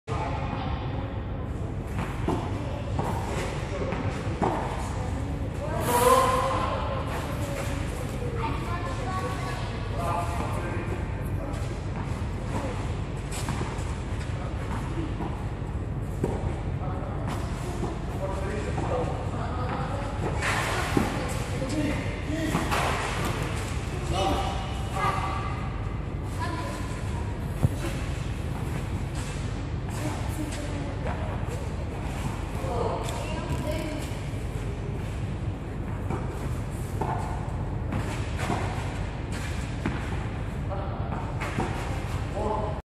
Play+Stay, Red - tennis play
Filmed on 19/04/2014 at Pro Tenis Vargolici venue in Bucharest, Romania.